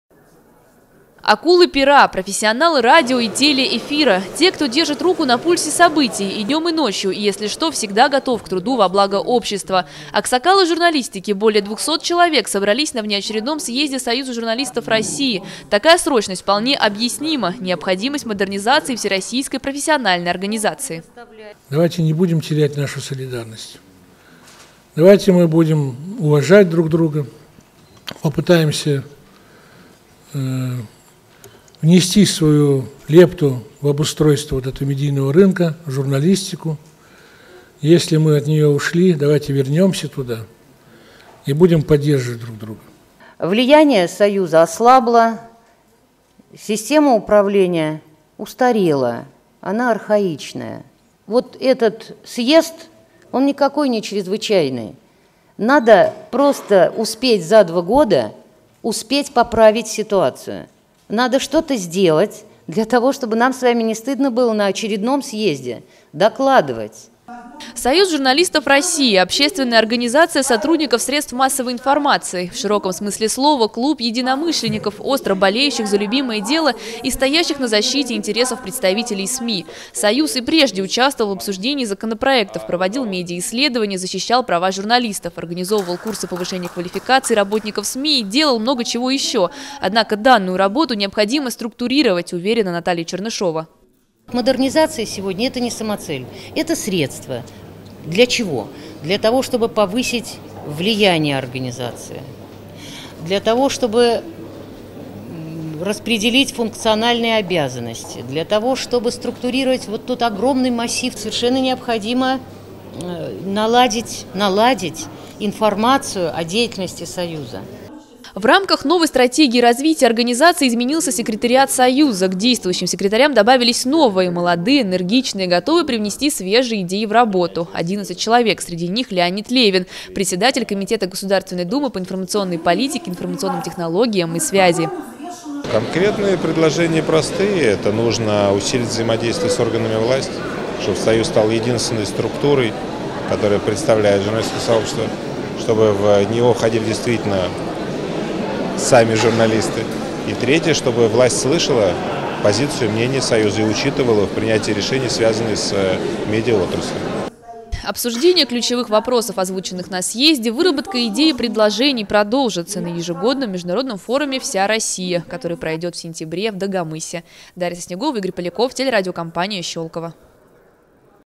Более 200 делегатов стали участниками внеочередного съезда Союза журналистов России
Более 200 делегатов из различных регионов страны стали участниками внеочередного съезда Союза журналистов России. На повестке дня – принятие программы модернизации всероссийской профессиональной организации и избрание в состав Союза новых секретарей. О результатах съезда -  в материале нашего корреспондента